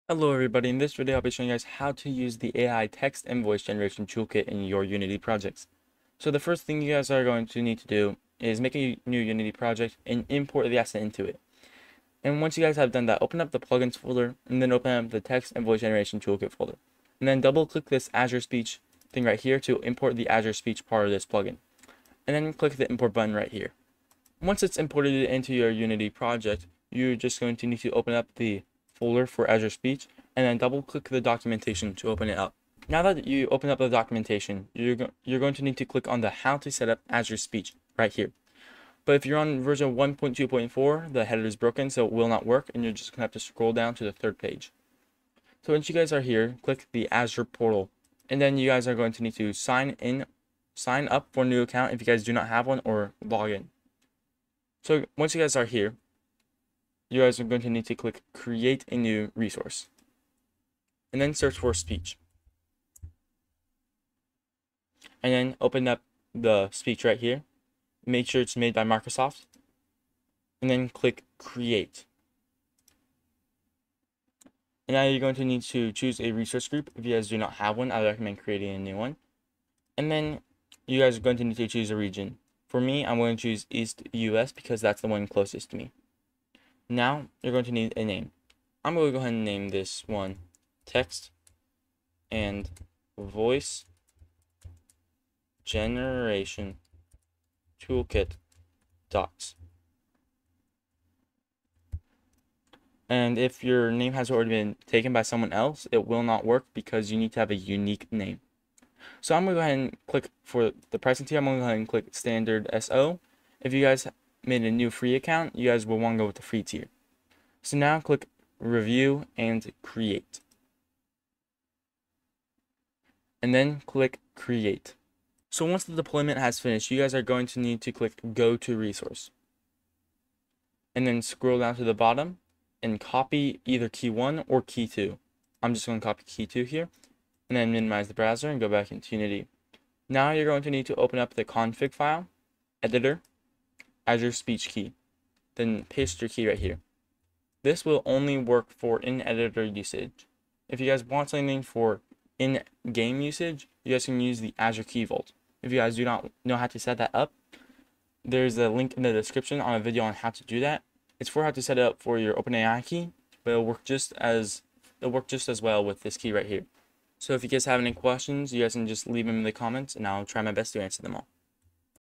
How to Set Up Azure Speech - AI Text And Voice Generation Toolkit
In this video, I will guide you through the process of setting up Azure Speech for the AI Text and Voice Generation Toolkit Unity asset. The AI Text and Voice Generation Toolkit is a powerful Unity asset that allows you to generate text and voice interactions in your Unity projects. You can find the AI Text and Voice

You can find the AI Text and Voice Generation Toolkit on the Unity Asset Store